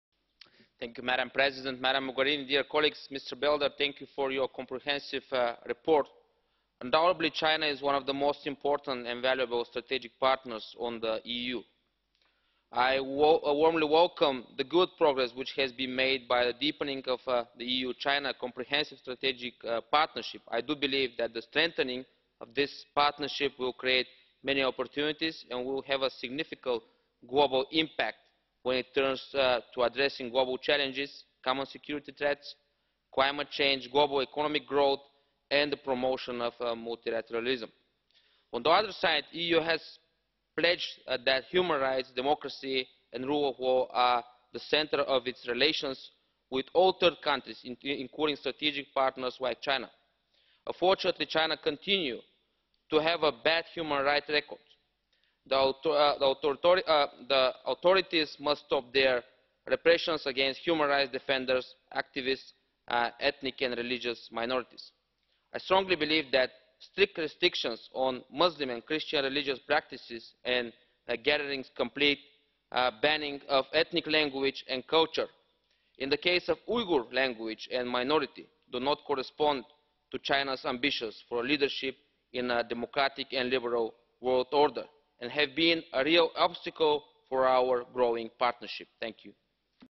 Ilhan Kyuchyuk 11 Sep 2018 plenary speech on EU China relations
Ilhan Kyuchyuk 11 Sep 2018 plenary speech on State of EU-China relations